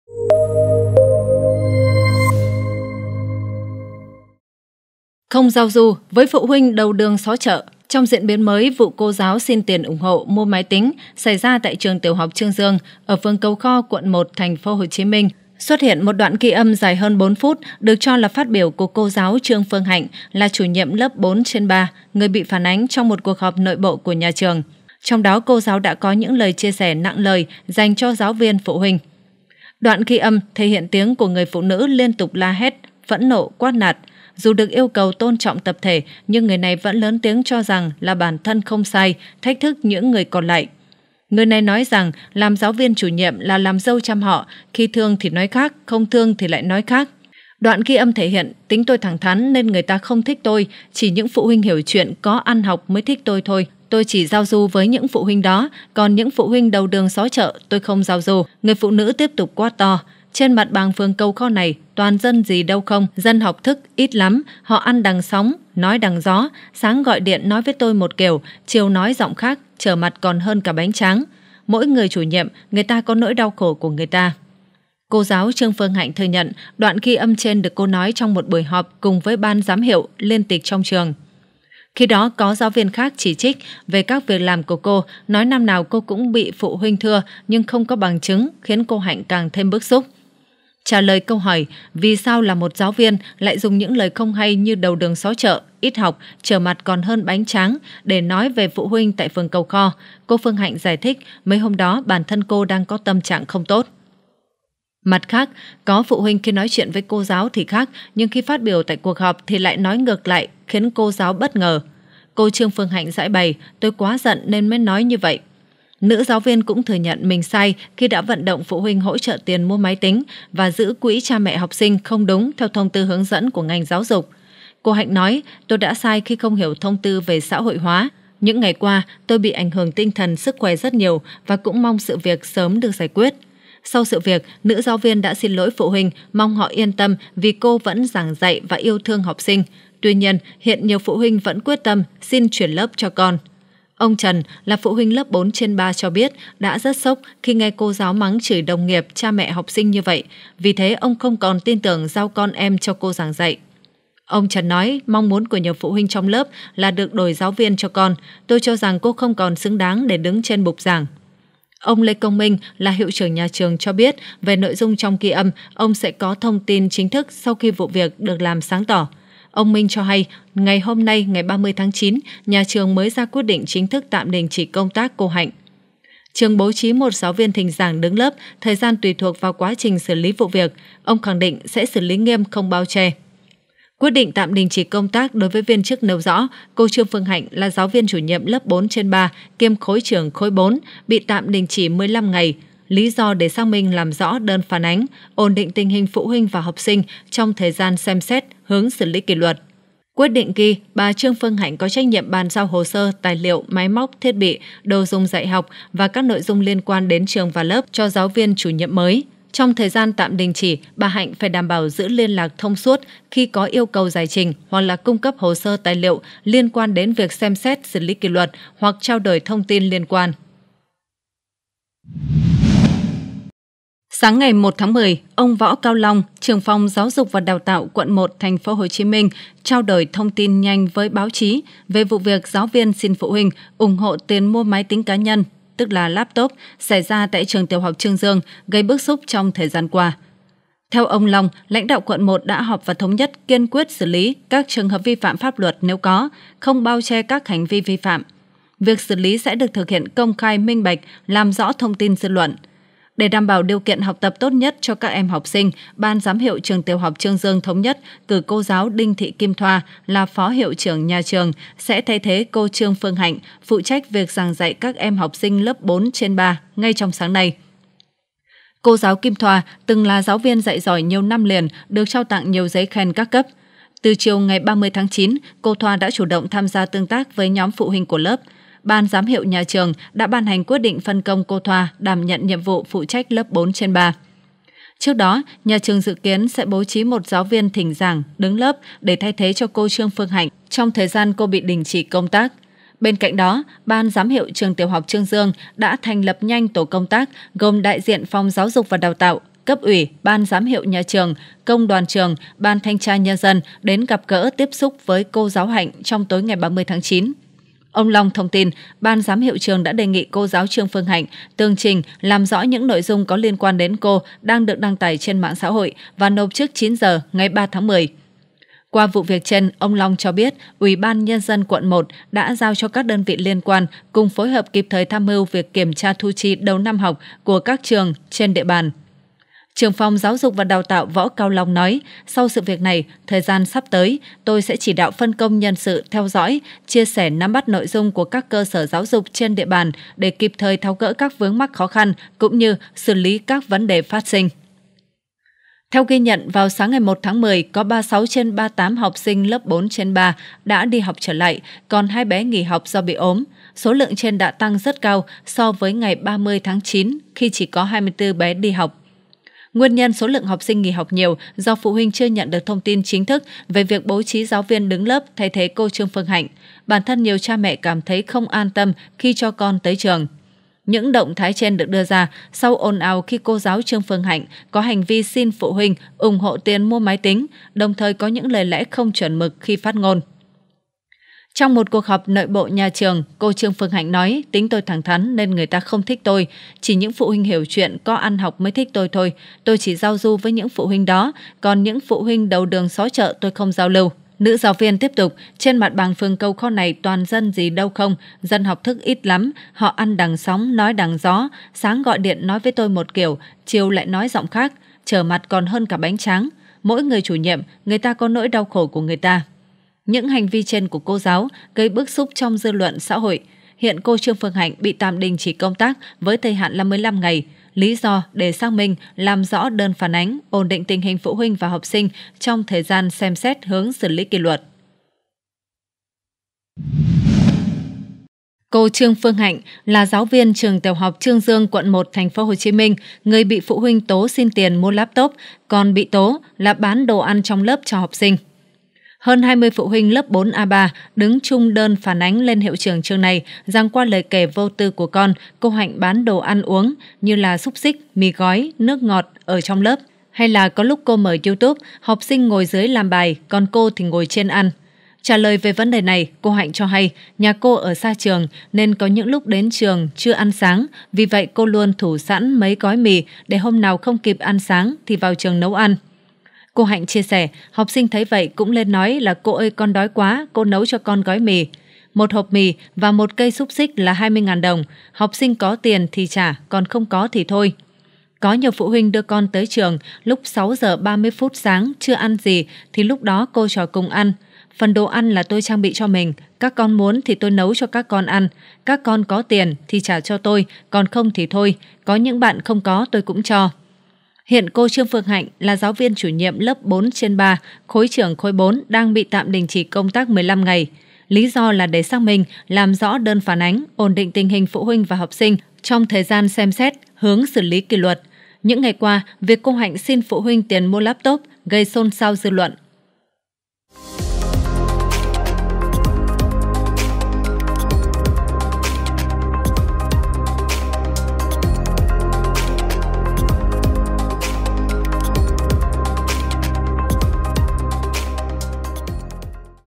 Cô giáo “xin tiền” mua laptop nói gì khi lộ đoạn ghi âm mắng phụ huynh "đầu đường xó chợ"? | VTs
Trong diễn biến mới của vụ cô giáo xin tiền ủng hộ mua máy tính xảy ra tại Trường Tiểu học Chương Dương (phường Cầu Kho, quận 1, TPHCM), xuất hiện một đoạn ghi âm dài hơn 4 phút mắng phụ huynh “đầu đường xó chợ”. Giải thích về vấn đề này, cô giáo trong vụ việc nói trên thừa nhận, trong lúc nóng giận đã có những lời lẽ chưa phù hợp khi nói về phụ huynh.